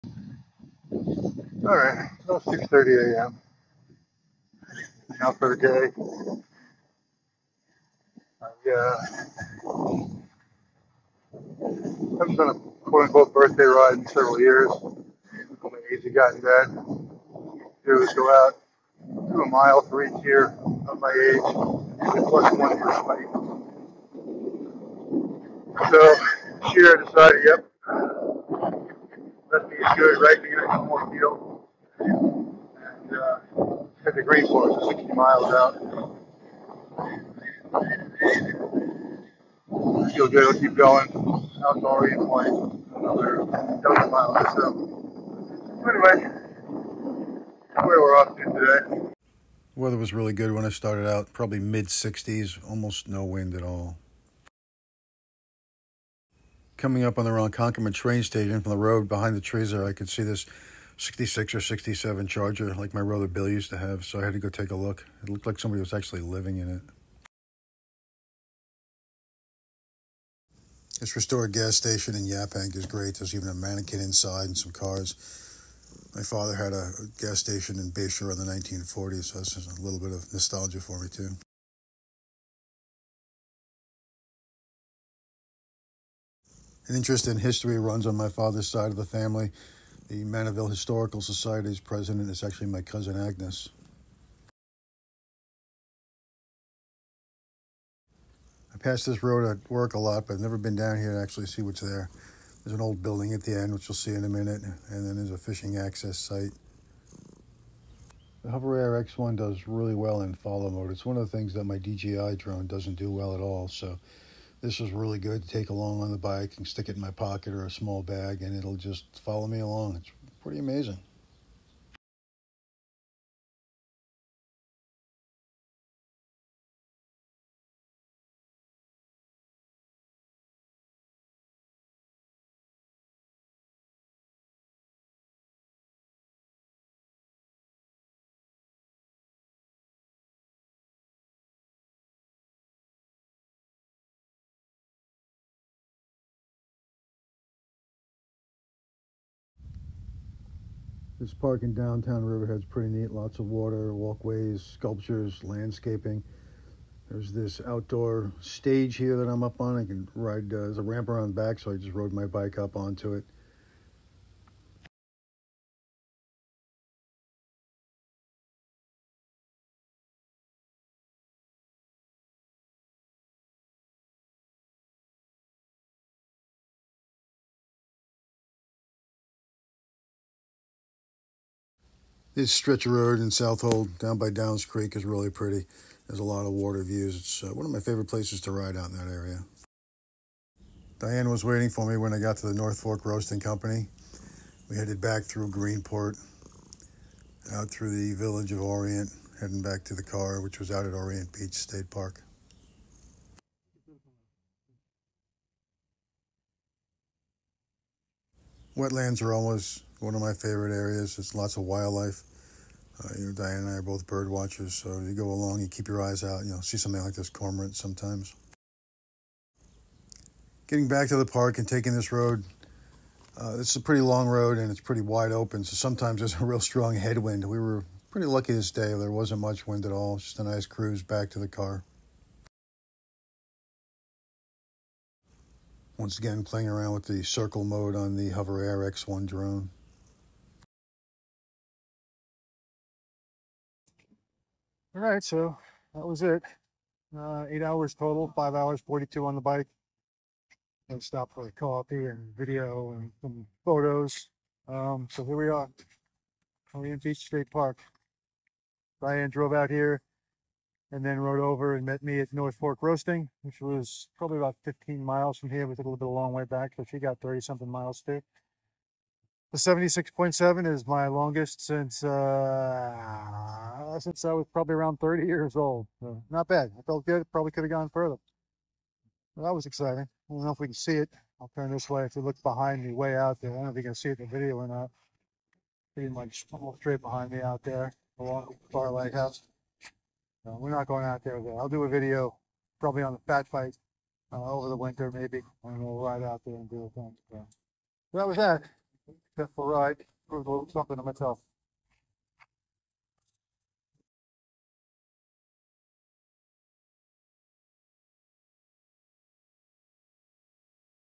Cycling from Islip Terrace to Orient Point. Sunday, July 28, 2024.
Here’s my first attempt at using CapCut to make a video, with stills from my iPhone and video from my new HoberAir X1. I took Monday off, downloaded the app, watched some tutorials, and made the video on my couch in a few hours.  Now that I have an idea what it takes, I have ideas on how to get better at it.

About the ride…

I rode from my hometown of Islip Terrace, NY to the tip of Long Island’s North Fork. 76.8 miles. It was my longest bike ride since my early 30s, and a practice run for a century ride I hope to do in October.

Diane drove out, rode her bike to meet me at a coffee shop in Southold, then rode the last 17 or so miles with me back to the car. She ended up with about 32 miles.

I’m going to try to make more videos of my cycling adventures - on road, trail, and beach - and include history, nature, etc.

Gear:

(It’s equipped with SPD pedals, ISM PR saddle, ICAN Aero40 carbon rims, 26c Pirelli PZero Road tires, and Cyclami TPU tubes.)

Gorewear Distance bib shorts (Yes, they’re expensive, but they make a huge difference for my delicate derrière, and I got a good discount on them):

Aerotech cycling apparel (They are made in Pittsburgh, and have sizes for larger folk):